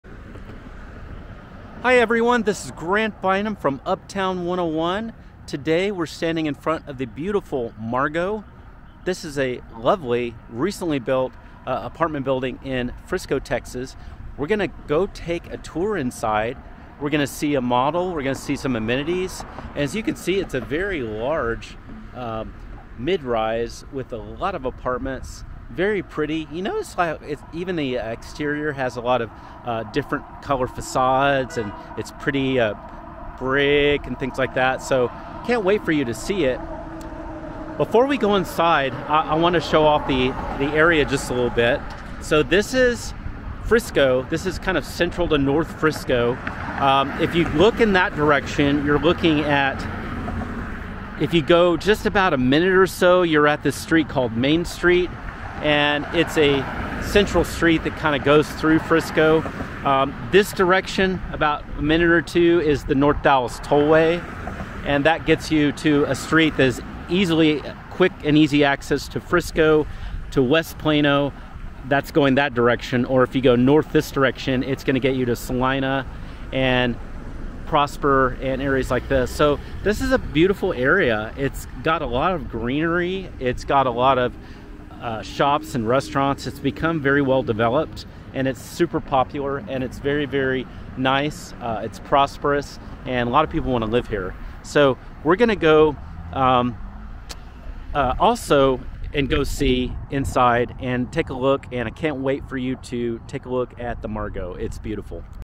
The Margo | Let's Tour It!
The Margo | Let's Tour It! This is a recently built and beautiful Frisco TX apartment!  It feels like a custom luxury home more than an apartment. It also has some of the best amenities I've seen - check out the pools and athletic center!

Ready to find the best Frisco TX apartments?
Use our 100%  free apartment service to save time and remove your stress!